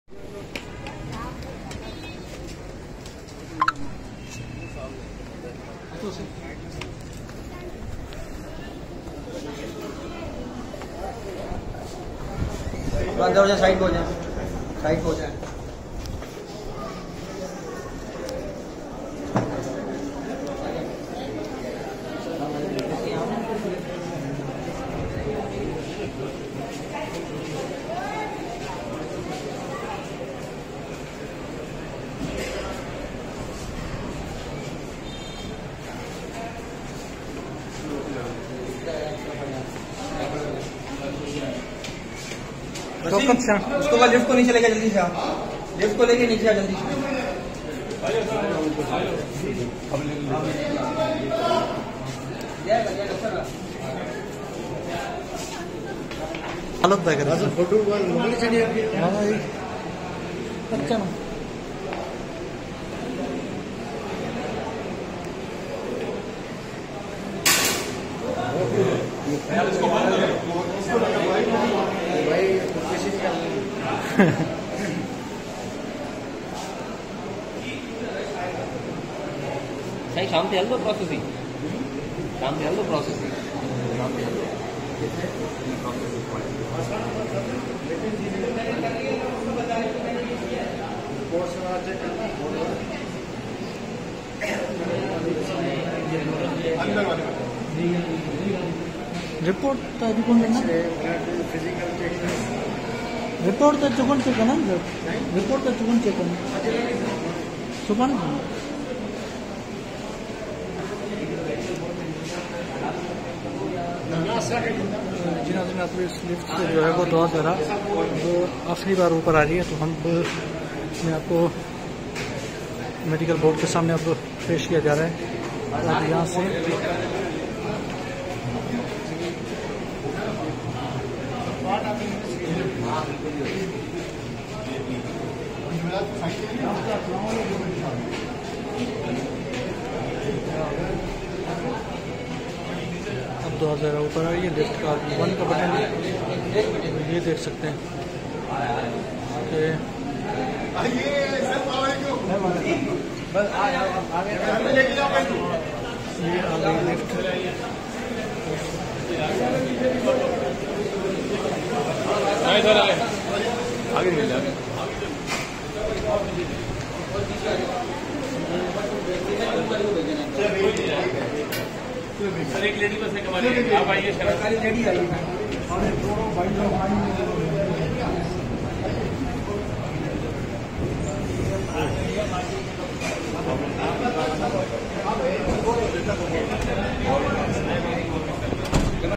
Dua Zehra all test mukmil kur diye gae hain Board k samny paish akhri dufa.
Zehra kazmi arriving in the hospital for medical test.